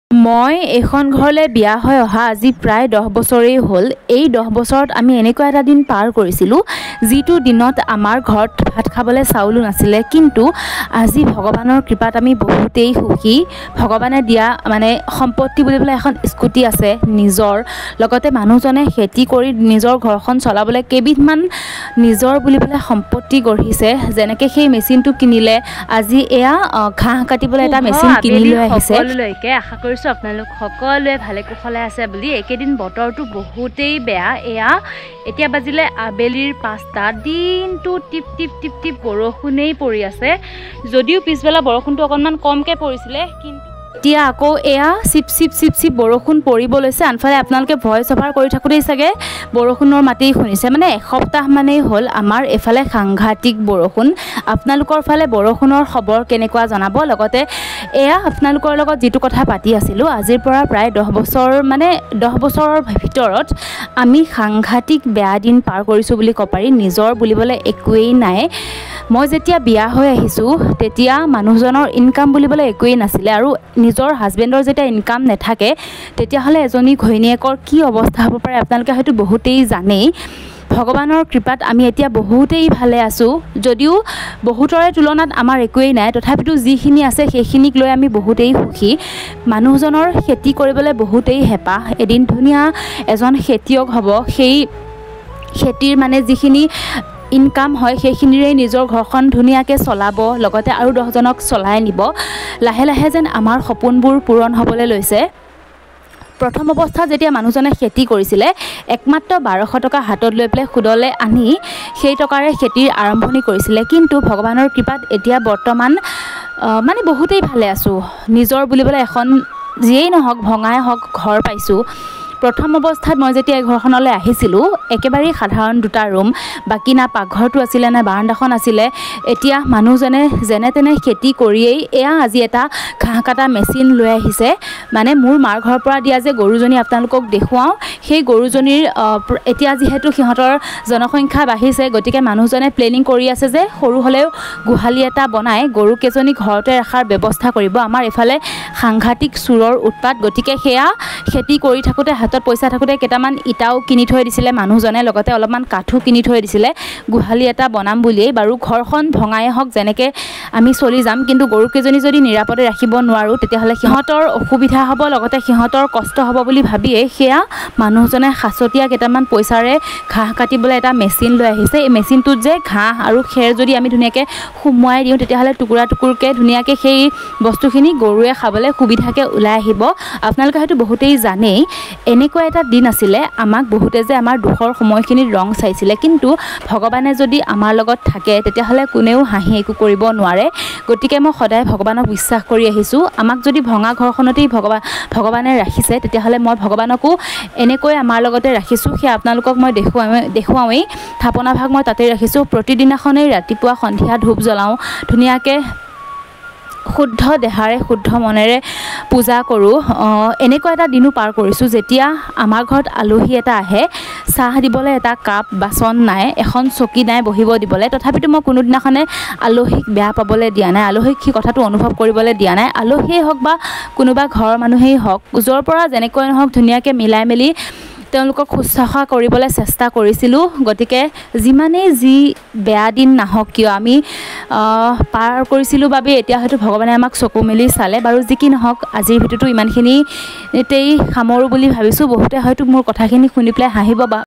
আমাৰ দুখৰ দিনত বহুতে আমাক ৰং চালে।।ঘৰত ভাত খাবলে চাউল নাছিল।
দুখৰ দিনত বহুতে ৰং চালে।viralvlogs vlog assamesevlogsvedio gitanjali vlogs#gitanjali VLOG#dailyvlog